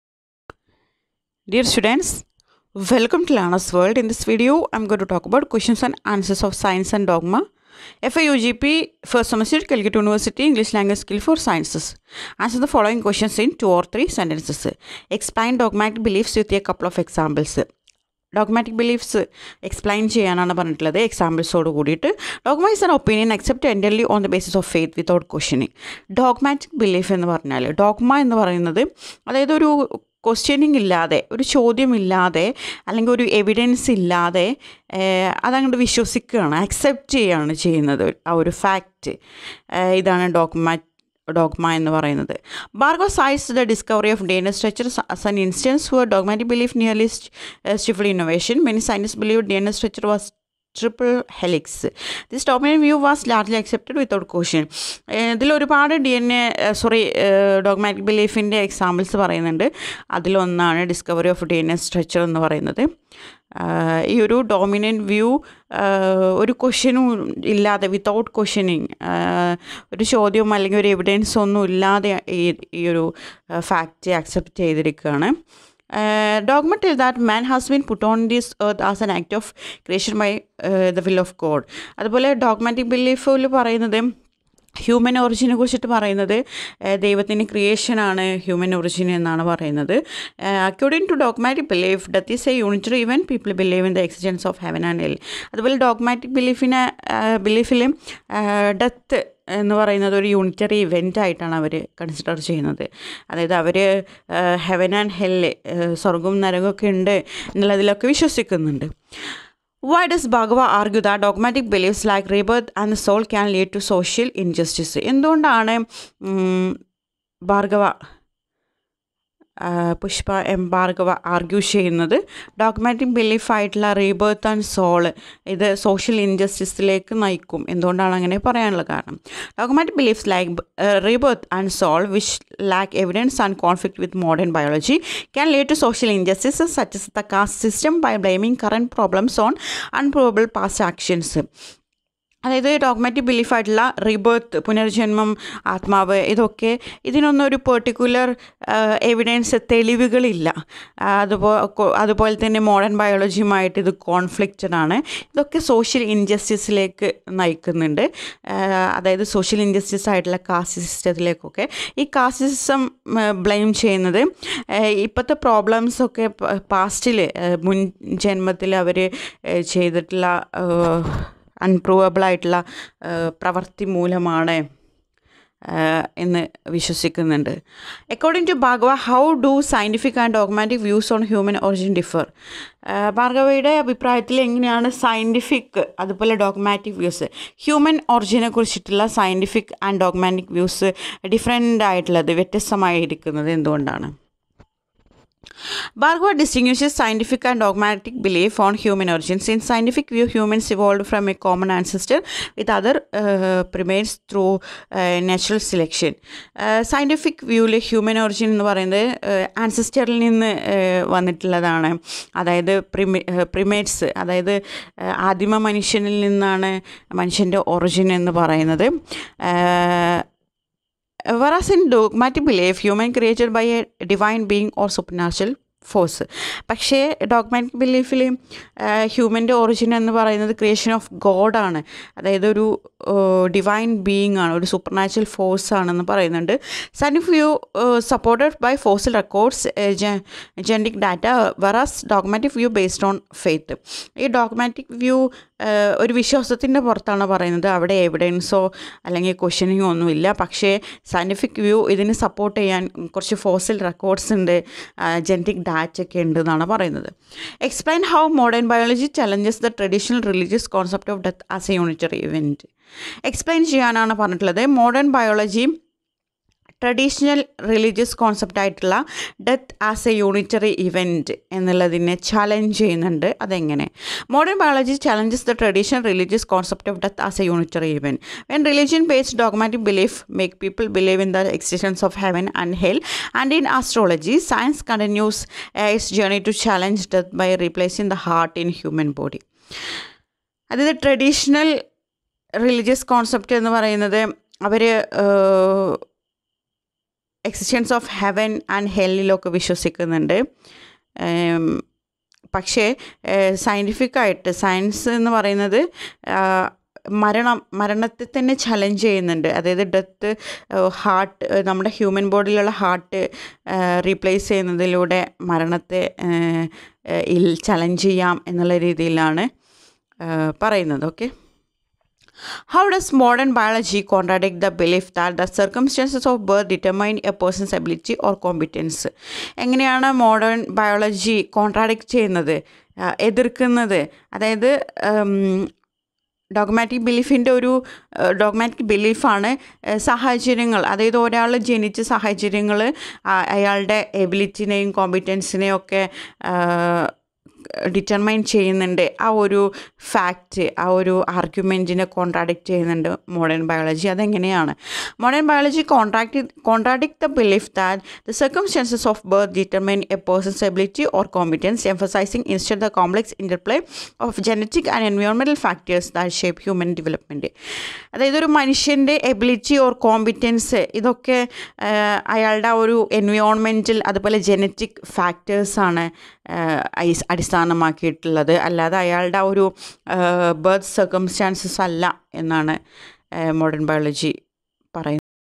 Questions And Answers Of Science And Dogma by Pushpa M Bhargava. Fyugp English for Science.
Questions And Answers of Science and dogma by Pushpa M Bhargava. Fyugp AEC English Language skills for science. First semester calicut University